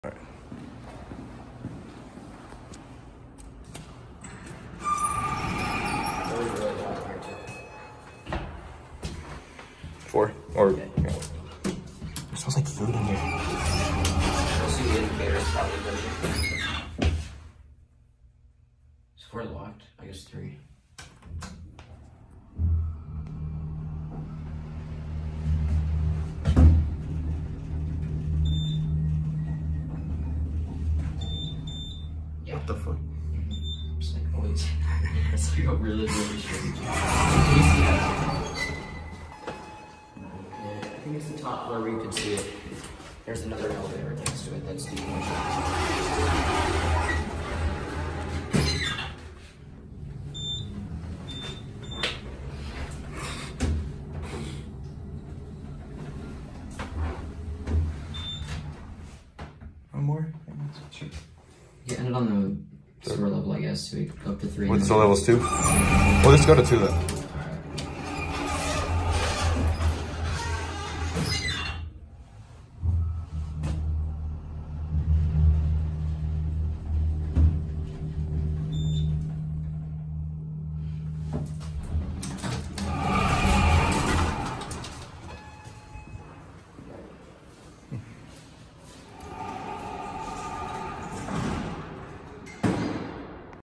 Montgomery Hydraulic Elevator, Macys Home, Westfield Montgomery, Bethesda, MD
Technical Specs:

Location: Macys Home, Westfield Montgomery, Bethesda, MD
Date Filmed: 3/25/24
Filmed on: iPhone 15 Pro Max

Elevator Specs:

Type: Traction
Year: 1974
Brand: Montgomery
Fixtures: PTL
Doors: Center Open

This was recorded during my March 2024 meet up with elevatorsofVA

This is the elevator at Macys Home, at Westfield Montgomery. If you’re wondering why the call button is so far away, there actually used to be second car to the right of this one, when this store was originally a Woodward & Lothrop. An AMC theaters was built on top of the garage, which is why the 4th floor is locked. I’m guessing these elevators didn’t get much traffic, so there was no point in having 2 elevators. As you can see, the indicator is very glitchy lol.